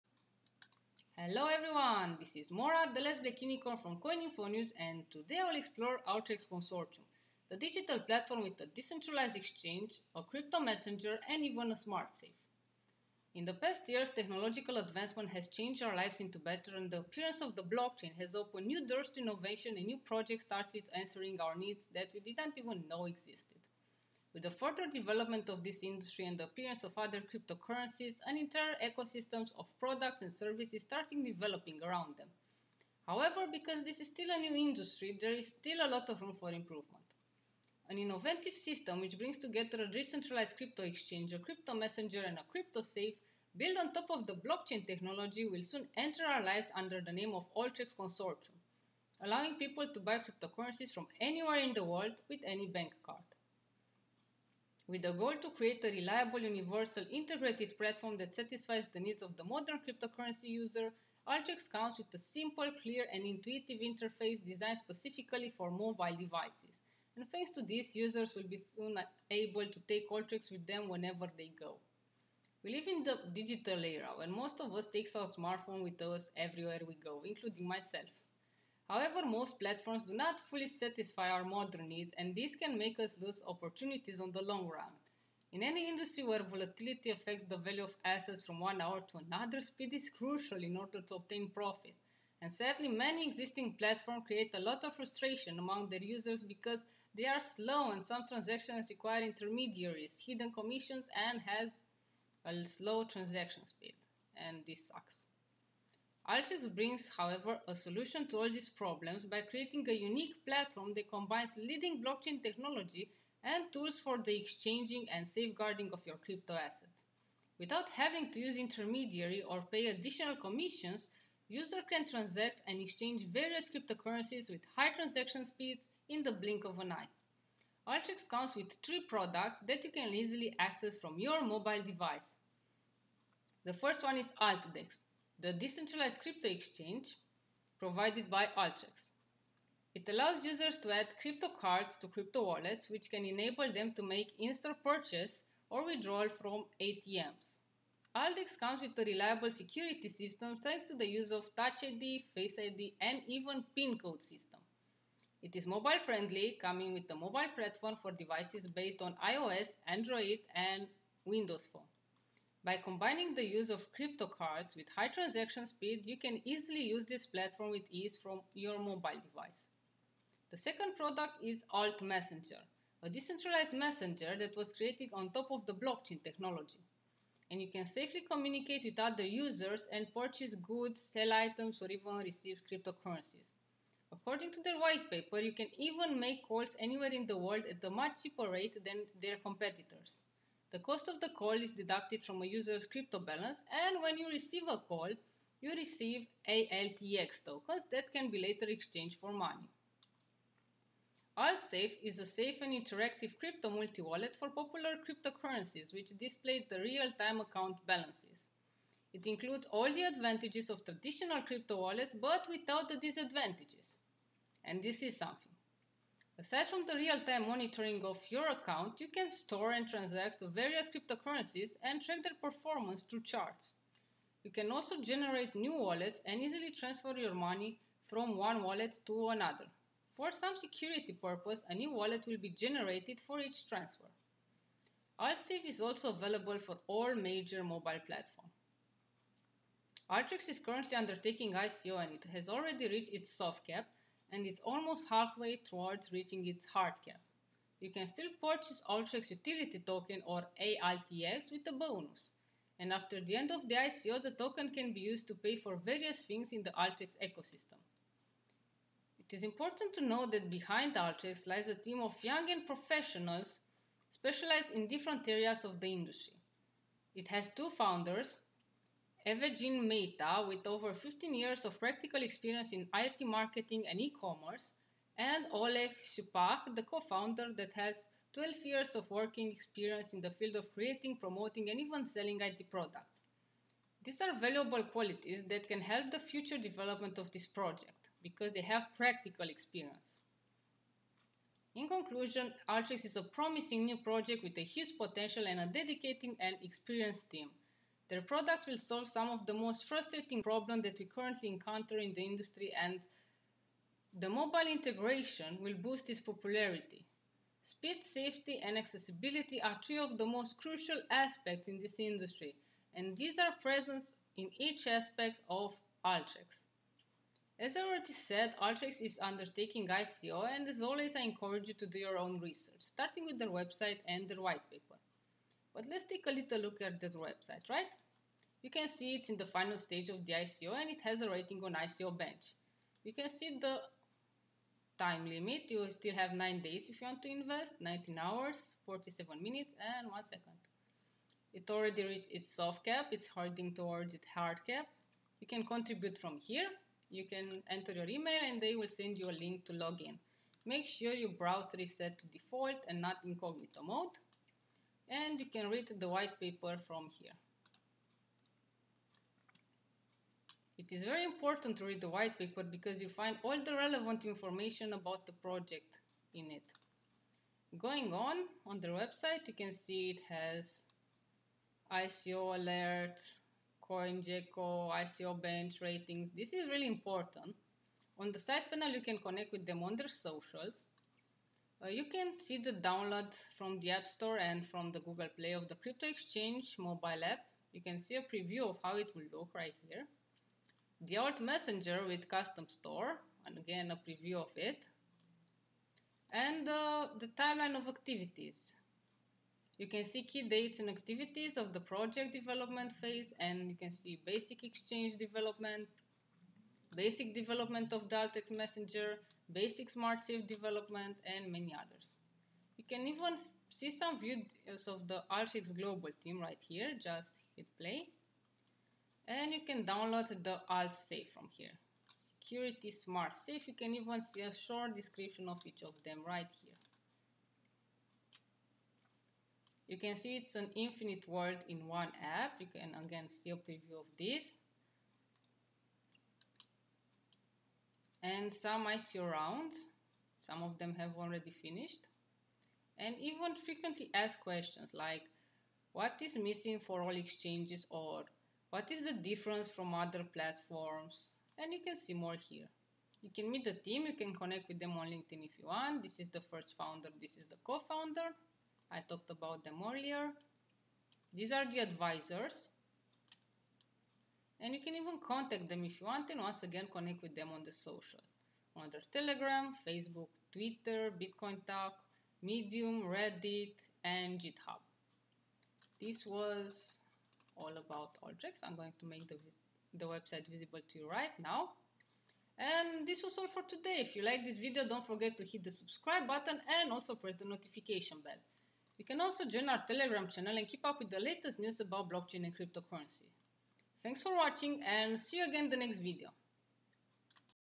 Alttex Consortium ICO Review - CoinInfo.News
Hello everyone! This is Maura, the Last Black Unicorn from Coin Info News! Today, I will explore Alttex Consortium, the digital platform with a decentralized exchange, personalized crypto-messenger and a Smart Safe.
With the goal to create a reliable universal integrated platform that satisfies the needs of the modern cryptocurrency user, Alttex comes with a simple, clear and intuitive interface, designed specifically for mobile devices. Thanks to this, users will be able to take Alttex with them, wherever they go.